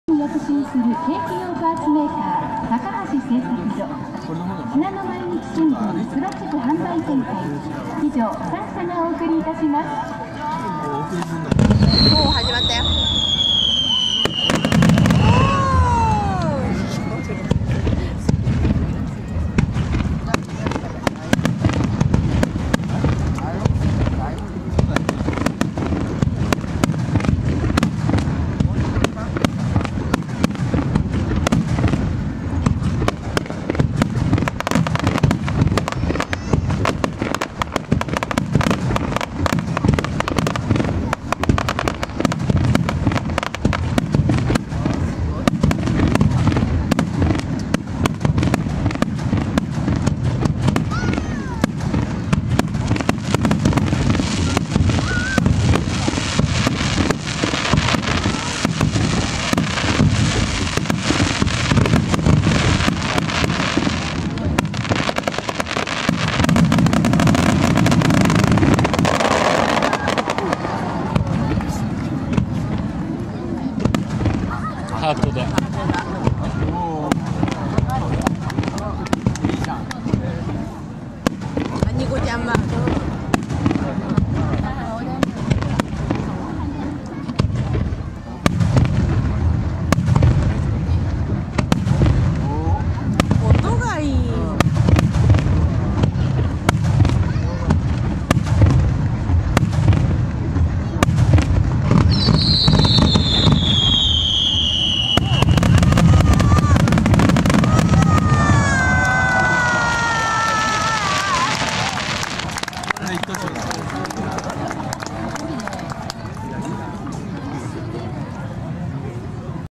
11世界へ羽ばたけPEACE DOVE
第68回　諏訪湖まつり　湖上花火大会　プログラム１１番　世界へ羽ばたけPEACE DOVE